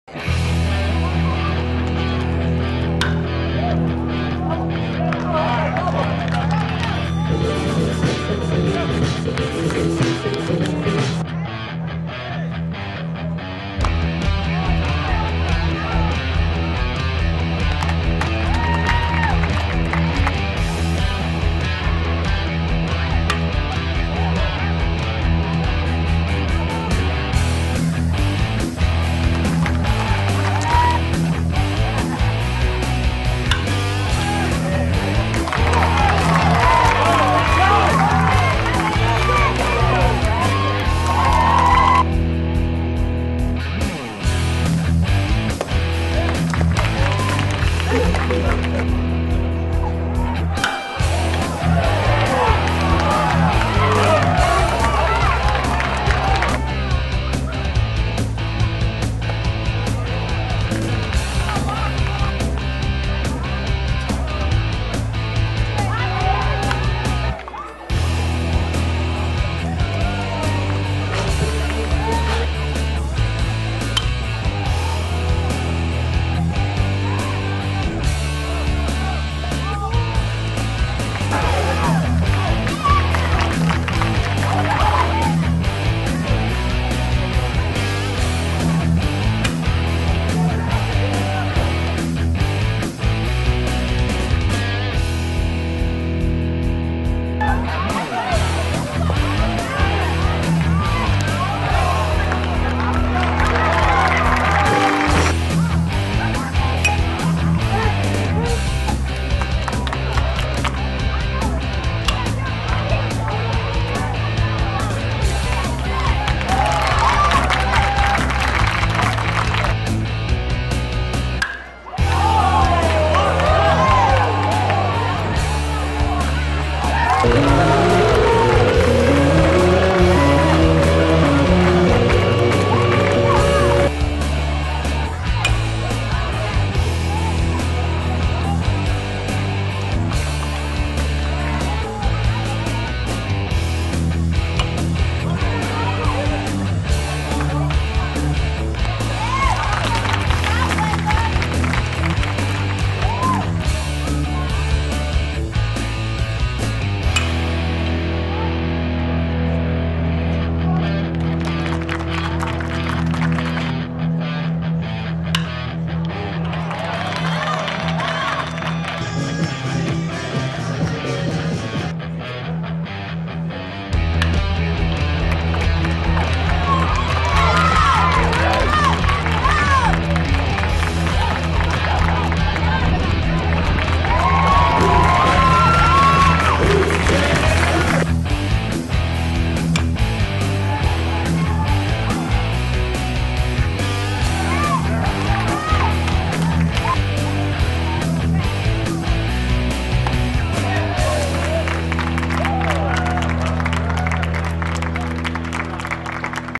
Blazer Baseball Highlights March 7 2012
Highlights from Blazer Baseball vs Georgia Southwestern on March 7, 2012